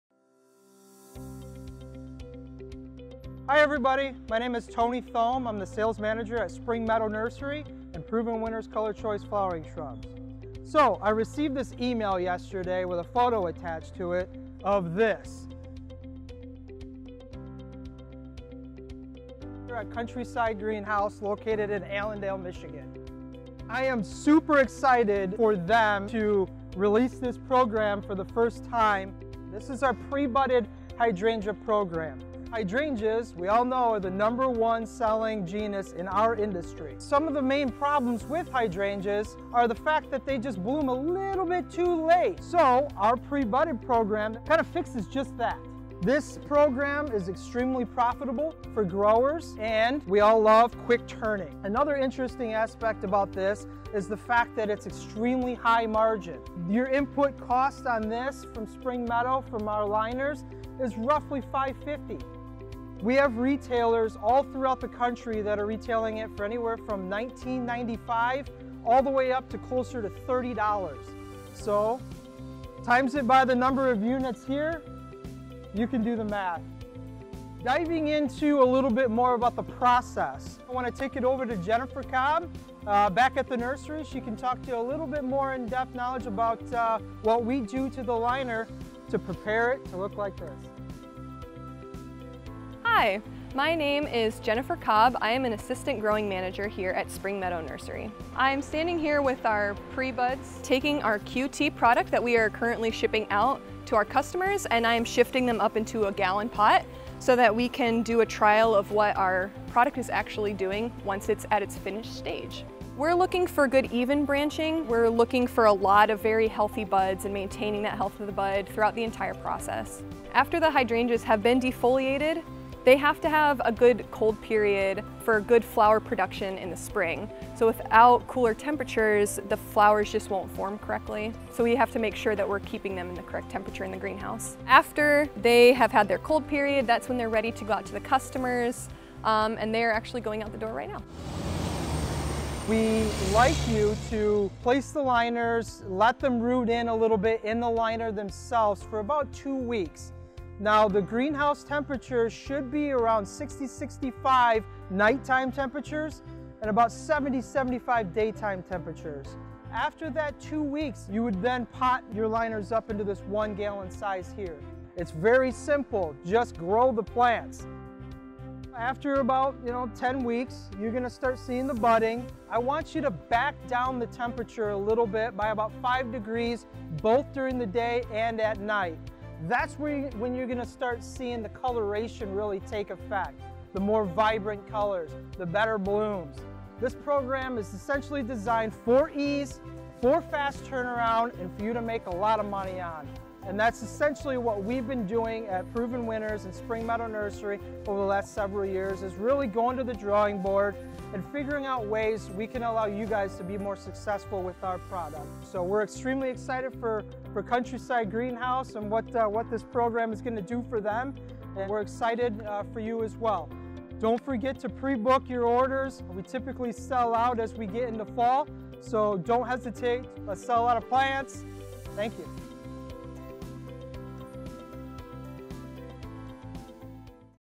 Pre-Budded Hydrangeas - Just in Time for Mother's Day!
Pre-budded hydrangeas from Spring Meadow are an easy way to grow a crop of colorful, in-bloom hydrangeas in time for Mother's Day.

Contact your account manager to reserve your plants today!